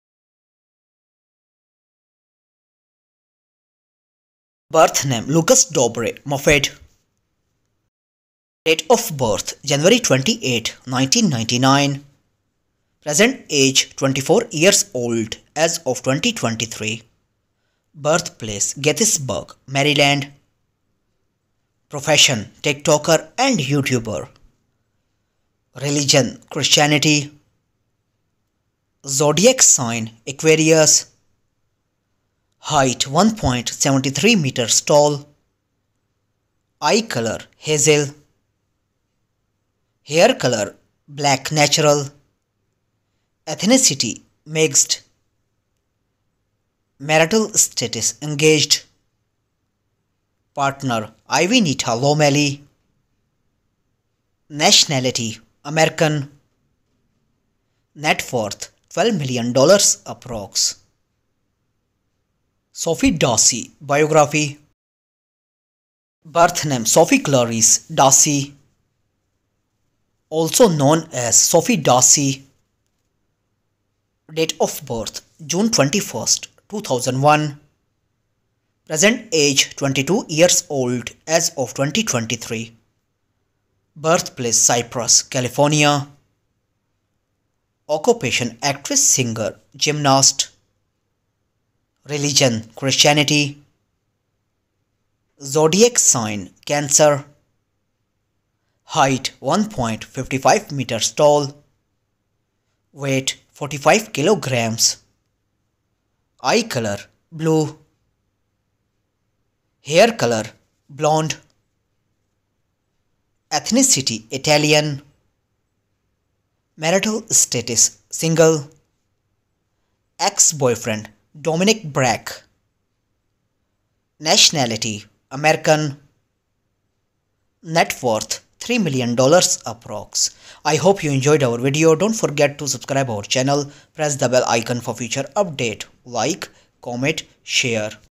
Lucas Dobre And Sofie Dossi Real Life Partner, Biography, Relationship, Net Worth, Hobbies, Facts
In this video, we delve into the lives of Lucas Dobre and Sofie Dossi, two talented individuals who have captured the hearts of millions with their incredible skills and charismatic personalities. Join us as we compare and contrast their real-life partnerships, explore their biographies, unravel the details of their relationship, and uncover fascinating facts about these popular influencers. Additionally, we'll delve into their net worth, hobbies, and other interesting aspects of their lives. Don't miss out on this exciting journey into the lives of Lucas Dobre and Sofie Dossi!